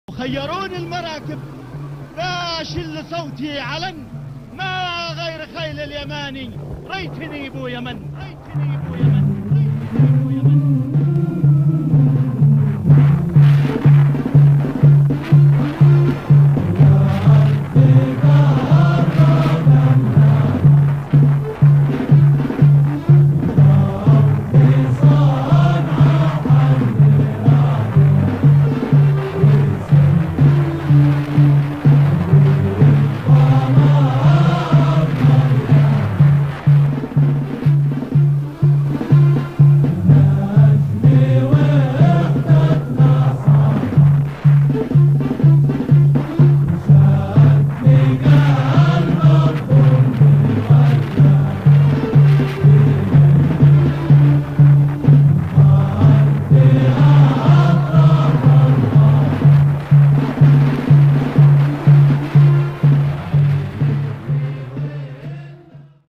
شاهد أبو يمن لما يحب يسافر من القرية إلى المدينة أو العكس
لقطة لإحدى سيارات عزلة خودان مديرية يريم محافظة إب وهي في طريقها إلى يريم، بعد أيام عيد الأضحى المبارك والناس يعودون لمزاولة أعمالهم في المدينة.